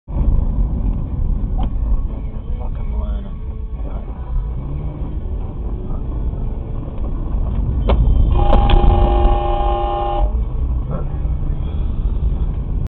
P plater nearly takes out bike
Speaks for itself, really. I'm pretty sure the Idiot in the white 4x4 didn't even see the bike.